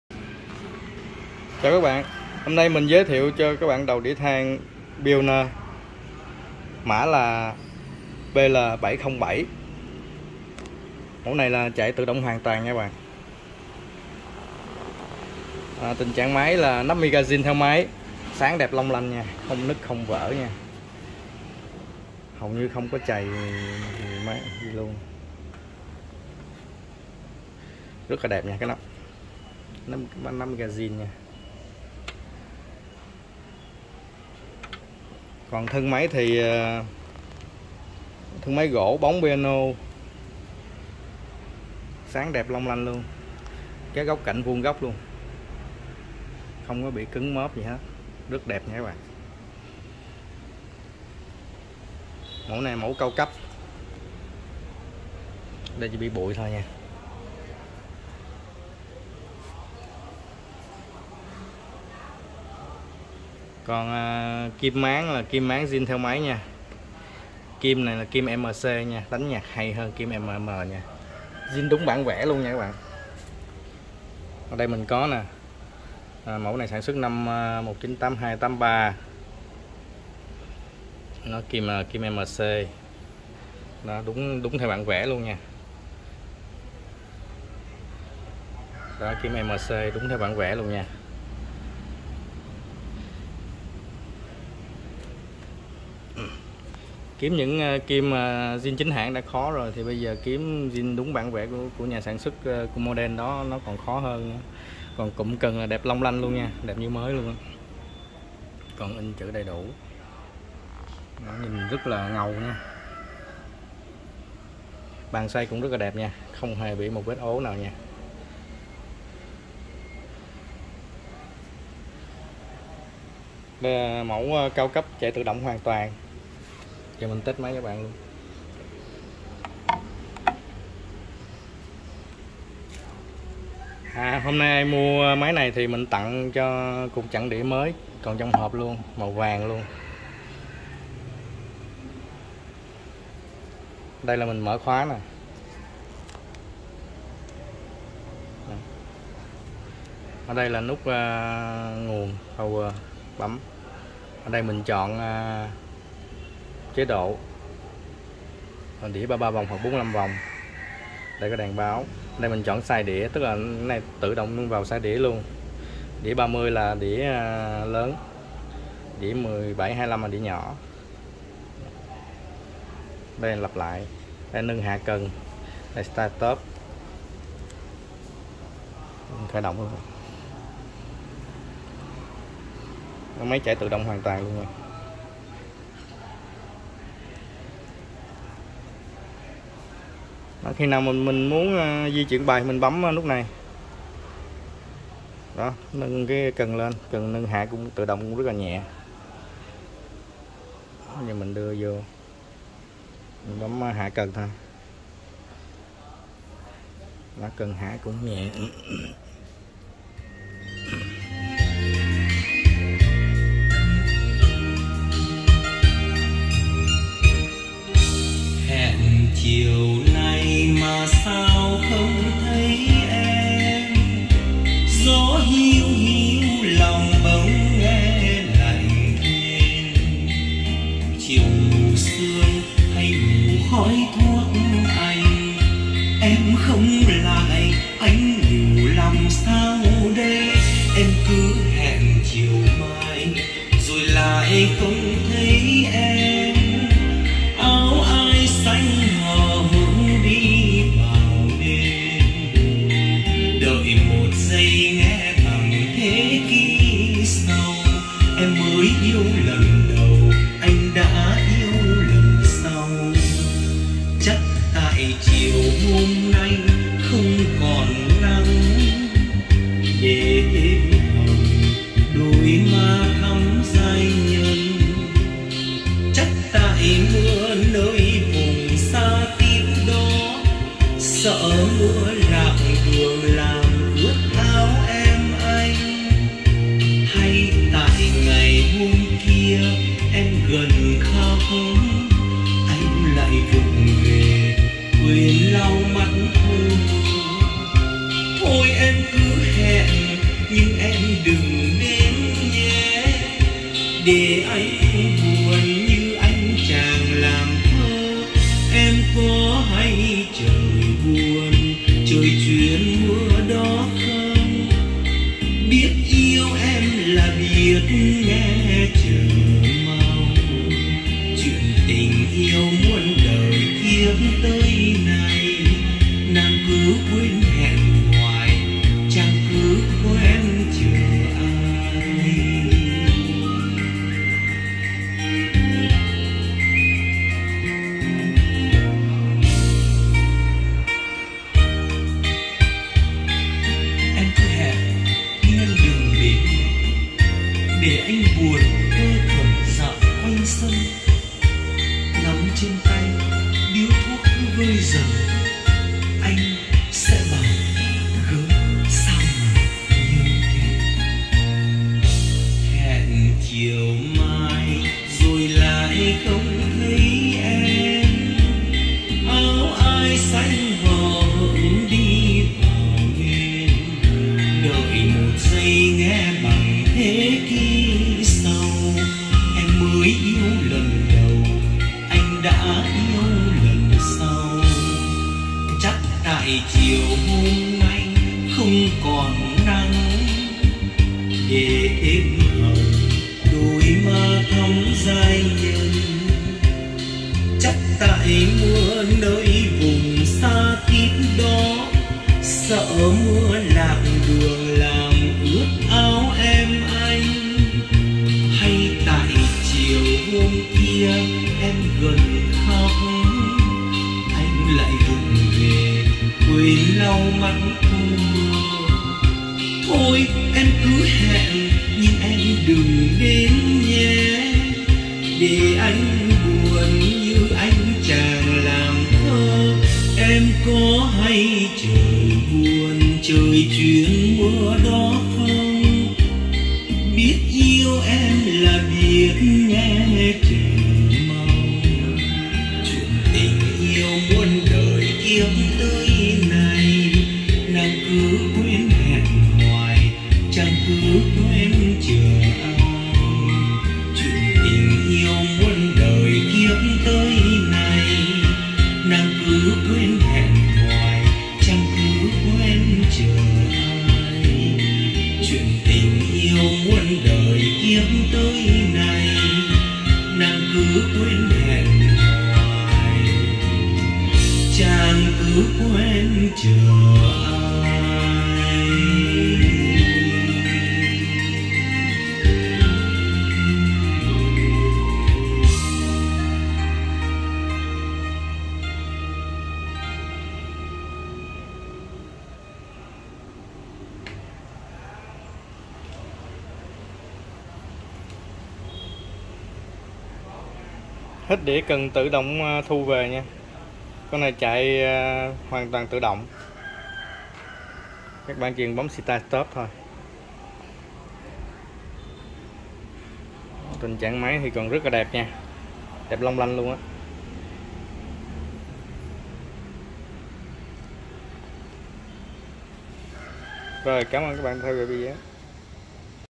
[ĐÃ BÁN]Đầu đĩa than Pioneer PL-707 Full automatic, Kim MC zin đẹp. Giá: XONG, lh 0908882918 Vinh SG
Đầu đĩa than Pioneer PL-707, chạy tự động hoàn toàn, kim MC, zin đẹp long lanh, sử dụng điện 100v
Tình trạng: thân máy gỗ bóng Piano zin nguyên bản từ nhà sản xuất đẹp long lanh
Kim máng zin theo máy, Kim MC pioneer zin đúng bản vẽ model
Nắp mika zin theo máy sáng đẹp long lanh.
💰ĐÃ BÁN (tặng 1 cục chặn đĩa), thanh toán chuyển khoản ship toàn quốc, hoặc qua nhà mua trực tiếp tại quận 5 SG, không ship COD
☎0️⃣9️⃣0️⃣8️⃣8️⃣8️⃣2️⃣9️⃣1️⃣8️⃣ Vinh SG Quận 5